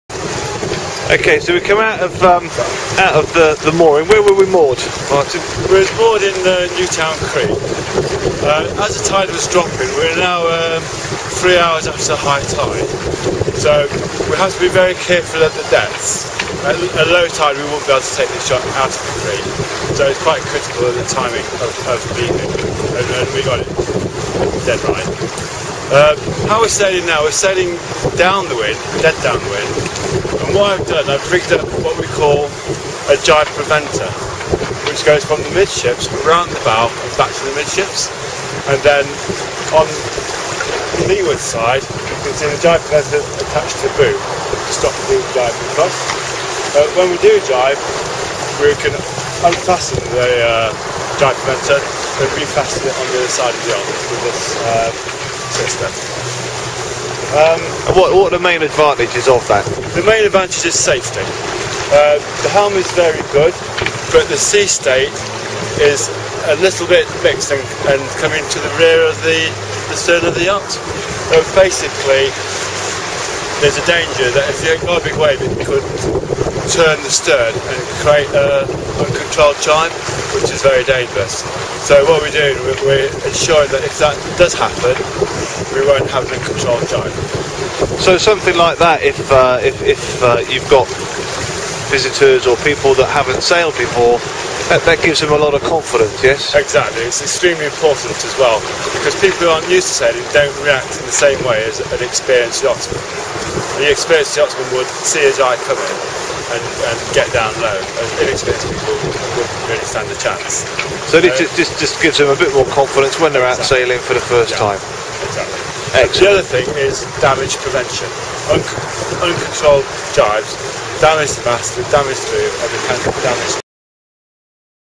Activesail Part 12_x264.mp4
Sailing after lunch, towards the Hamble River in the Solent. the Gybe preventer is explained.